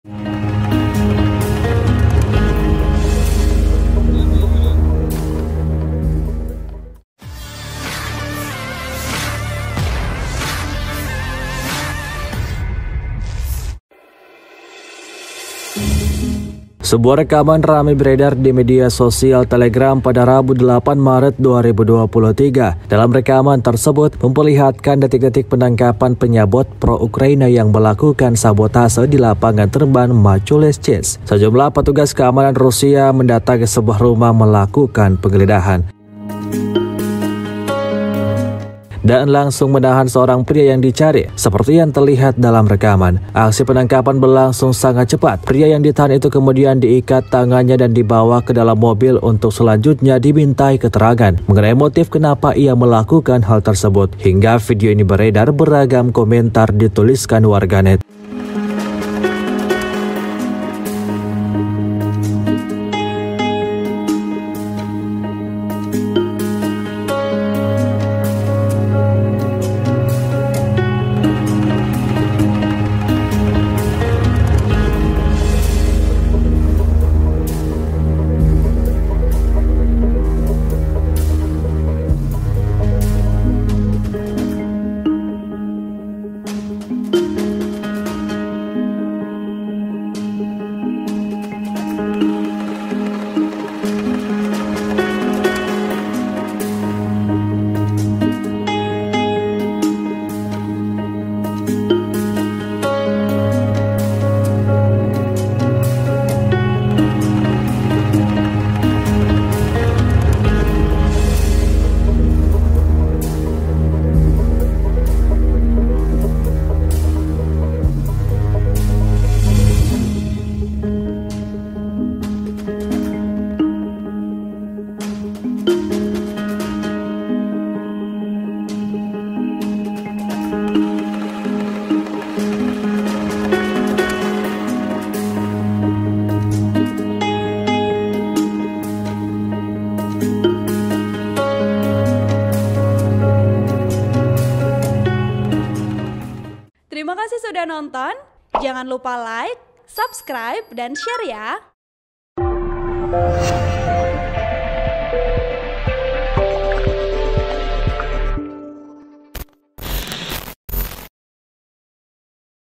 Detik-detik Penangkapan Penyabot Pro-Ukraina, Petugas Keamanan Rusia Sekap Tersangka
Dalam rekaman tersebut memperlihatkan detik-detik penangkapan penyabot pro-Ukraina yang melakukan sabotase di lapangan terbang Machulishchi.

Sejumlah petugas keamanan Rusia mendatangi sebuah rumah, melakukan penggeledahan.

Dan langsung menahan seorang pria yang dicari.

Aksi penangkapan berlangsung sangat cepat.(*)

Narator: Gunawan